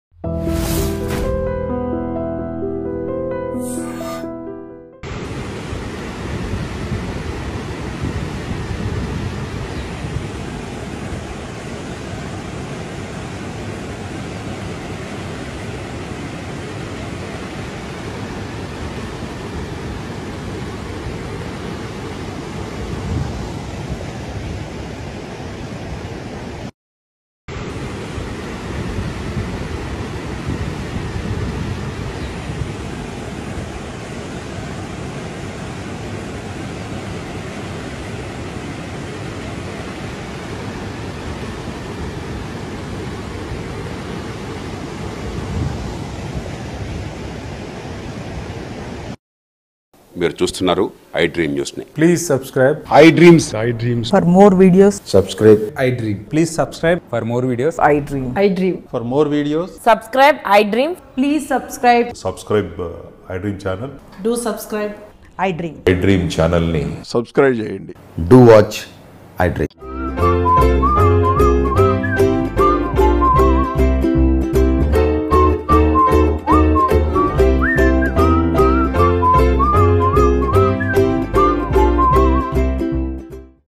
మూసారాంబాగ్ మరియు చాదర్‌ఘాట్ ఏరియా దగ్గర పొంగిపొర్లుతున్న మూసీనది || iDream Telangana
హైదరాబాద్‌లోని మూసారాంబాగ్ మరియు చాదర్‌ఘాట్ ఏరియా దగ్గర పొంగిపొర్లుతున్న మూసీనది వైమానిక దృశ్యం.. వరద నీరు సమీప ప్రాంతాలలోకి ప్రవేశించింది.